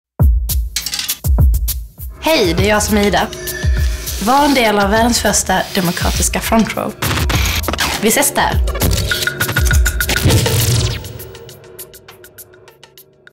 Tele2 och Ida Klamborn hälsar välkommen till
Idag smäller det! Du glömmer väl inte att ta din plats på front row under Ida Klamborns visning på Fashion Week Stockholm kl 17.15 direkt i mobilen?  Ladda ner appen Front Row: